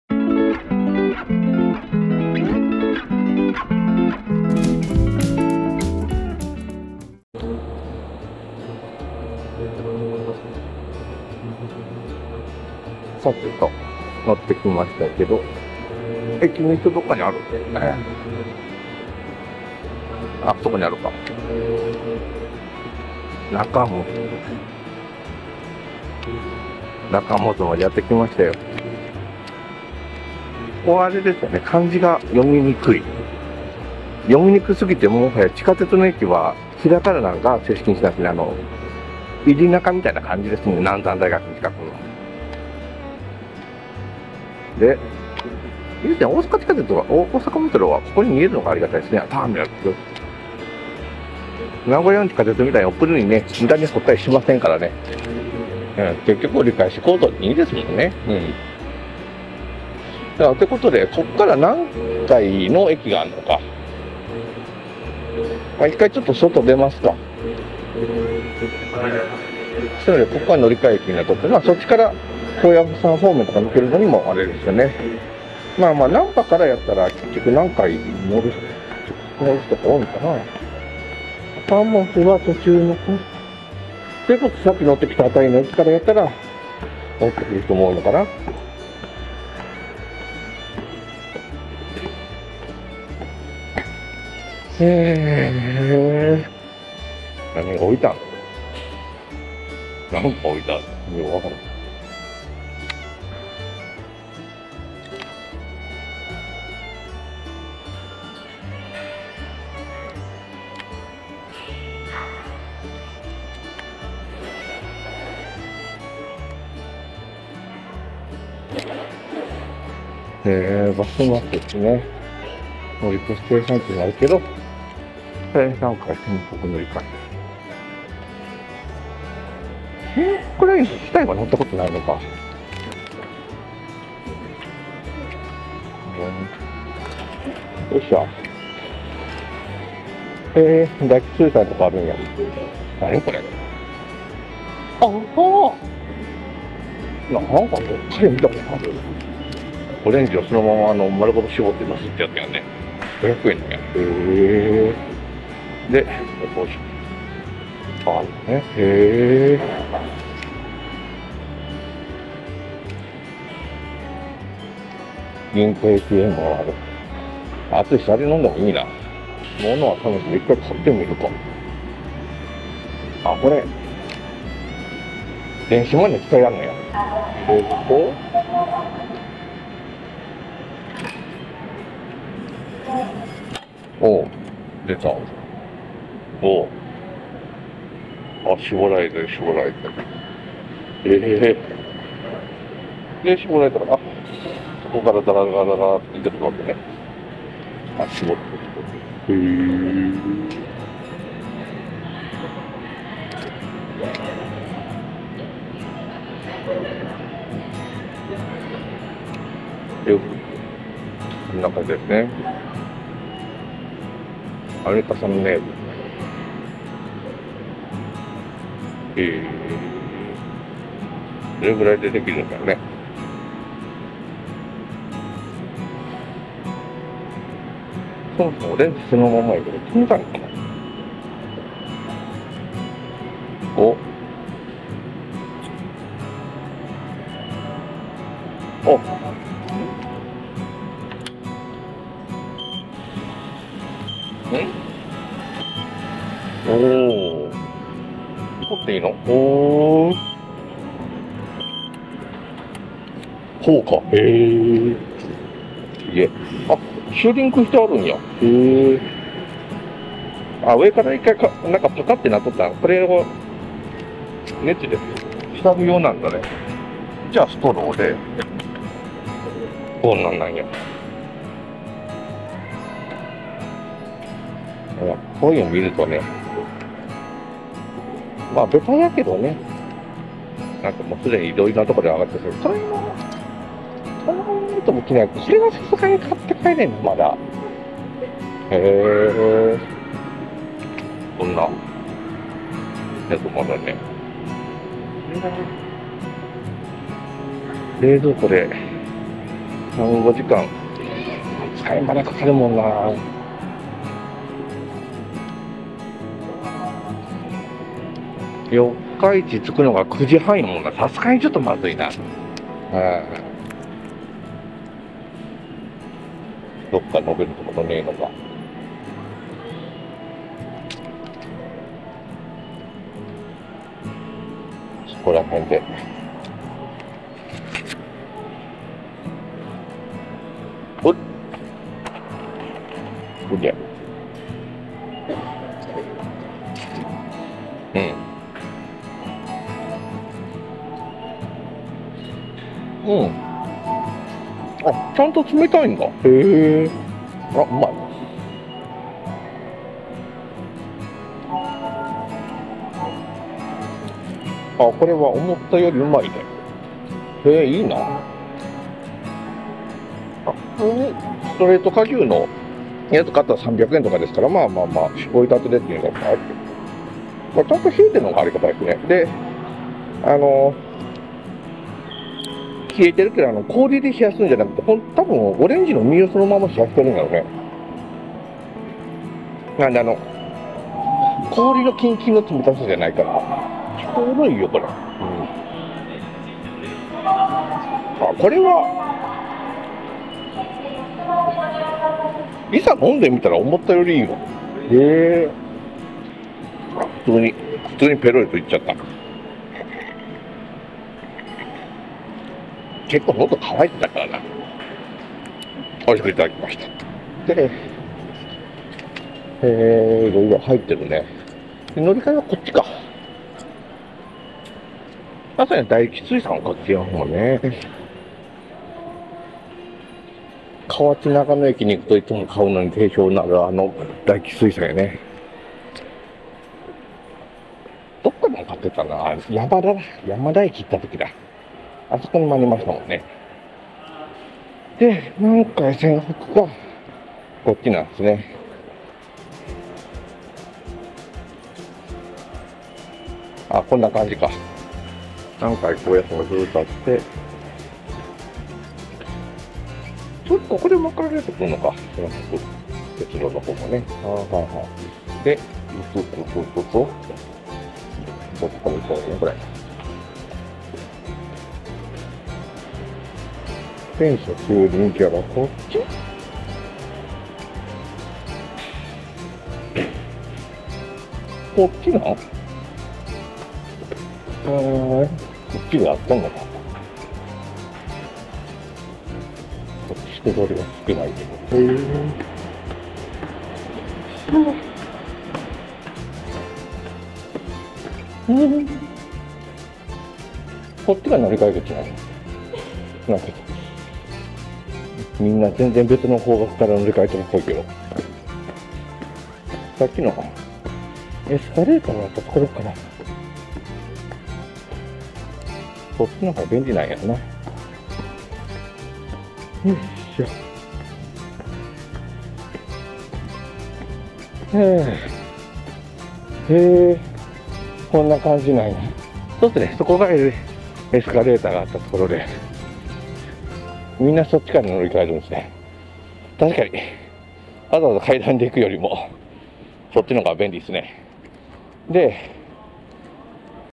ST125 ep17 末端駅制覇：なかもず駅＠堺市北区・御堂筋線【大阪メトロターミナル駅完全制覇の旅】
Location @googlemaps
〒591-8023 大阪府堺市北区中百舌鳥町２−２４０−１
大阪メトロ　御堂筋線/なかもず駅
2-240-1 Nakamozu-cho, Kita-ku, Sakai-shi, Osaka 591-8023
Osaka Metro Midosuji Line/Nakamozu Station

京阪・Osaka Metro　1日フリーチケット